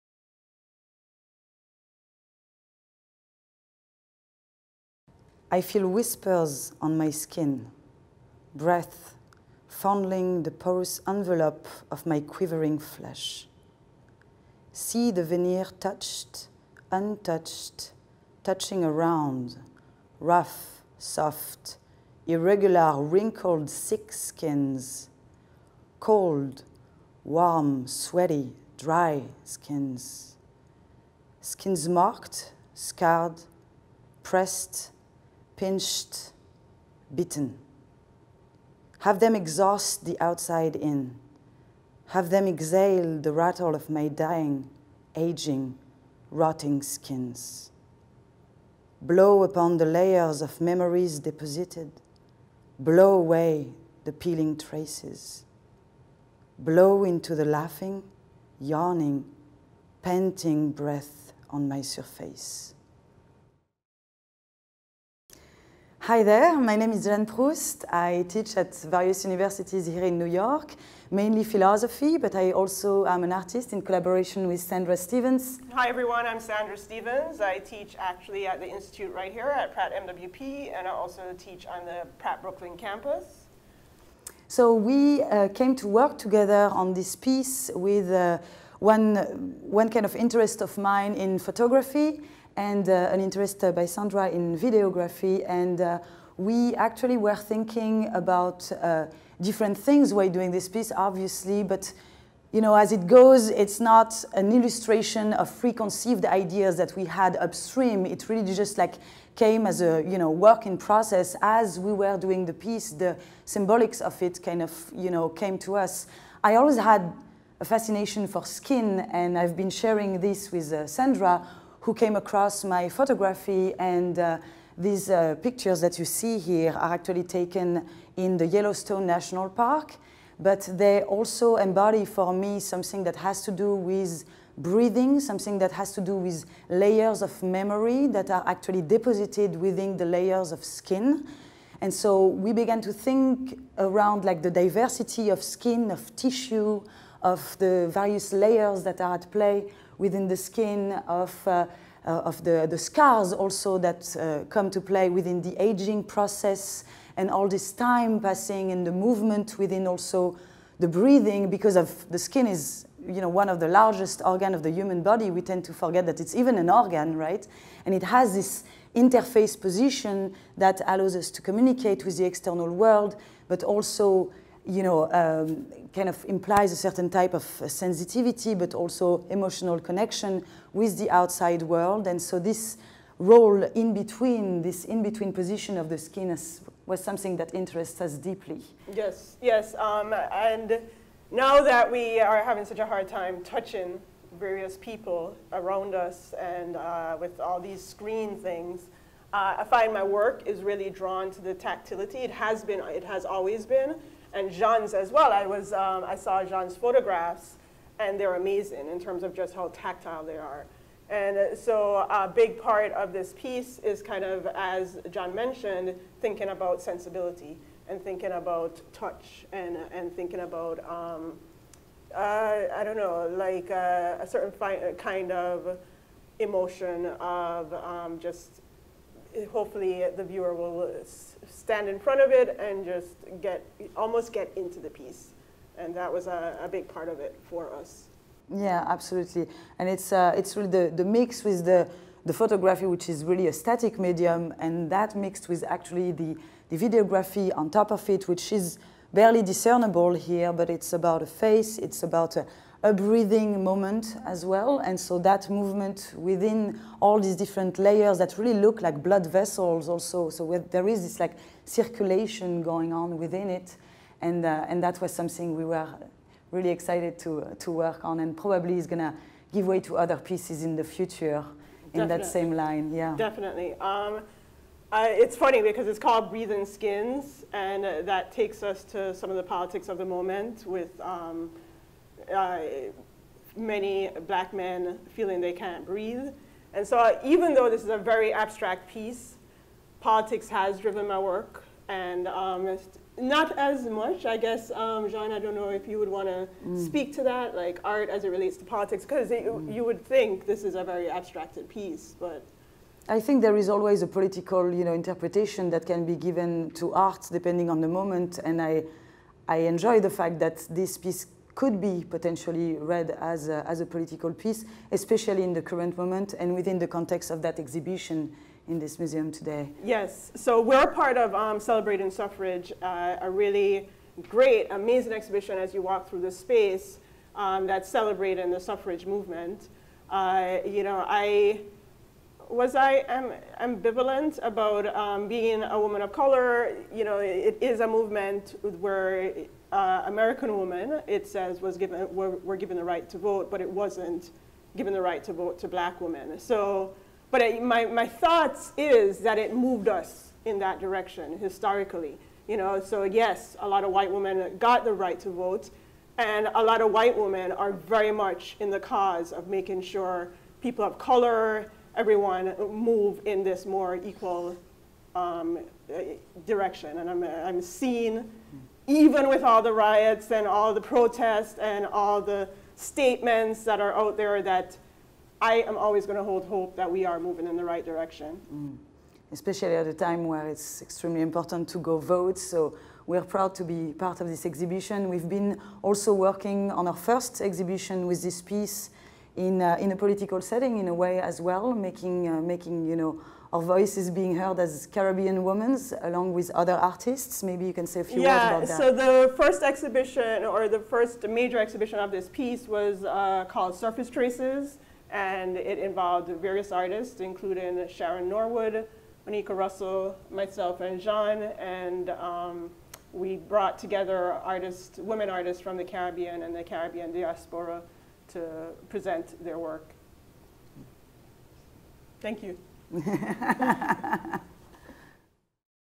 A Closer Look Talk: With Artists Sandra Stephens and Jeanne Proust
Learn about Breathing Skins, now on view in the exhibition Celebrating Suffrage. Breathing Skins is a collaboration between artists Sandra Stephens, PrattMWP Associate Professor, and Jeanne Proust.